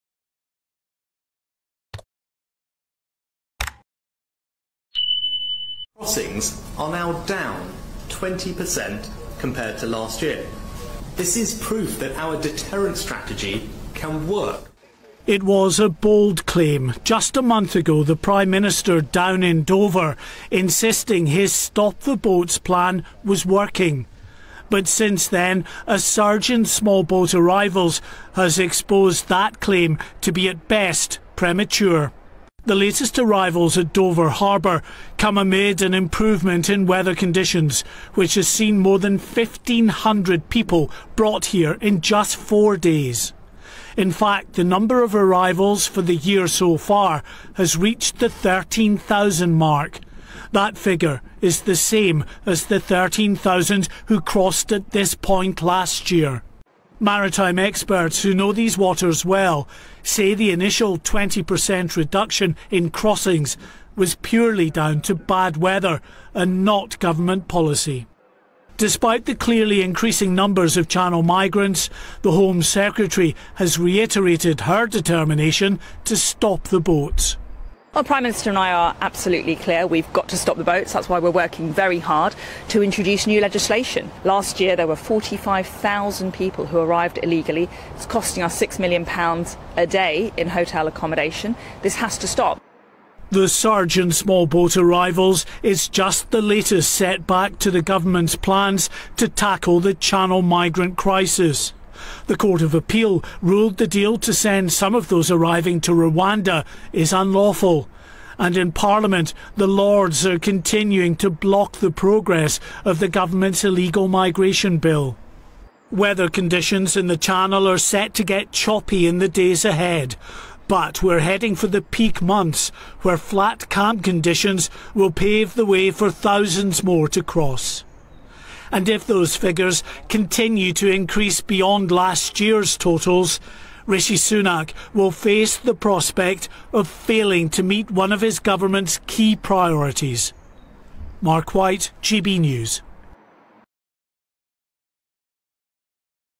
Surge in Channel crossings scupper 'stop the boats' pledge
‘A surge in small boat arrivals have exposed that claim to be at best premature.’

GB News’ Mark White investigates Government claims that their deterrent strategy for migrant crossings has had an impact on the ongoing crisis.